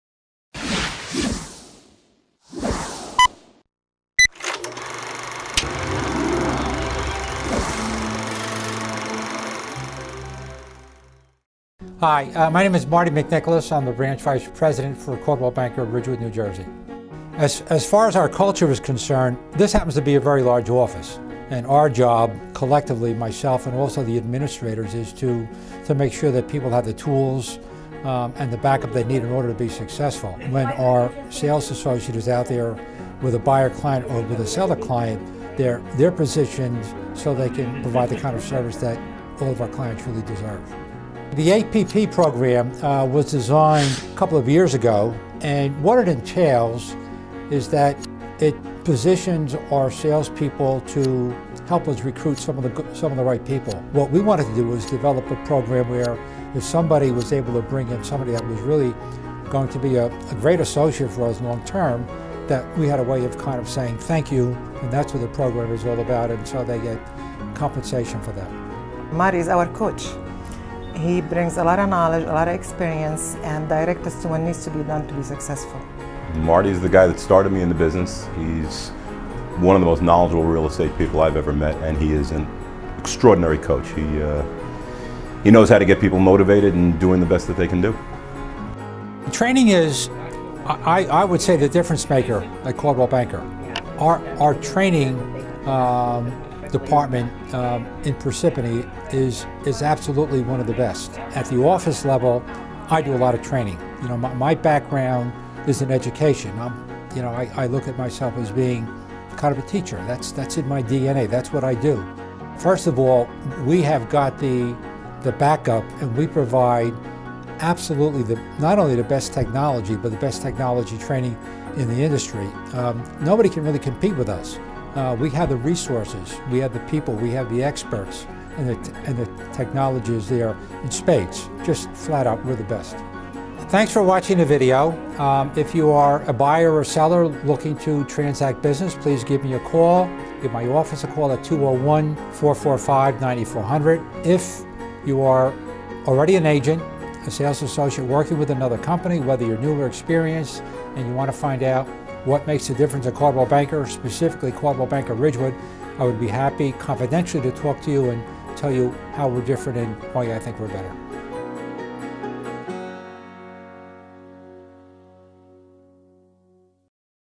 Marty McNicholas Video Profile
We would like to introduce Marty McNicholas of the Coldwell Banker Residential Brokerage Sales Office in Ridgewood, NJ.  Please enjoy the video.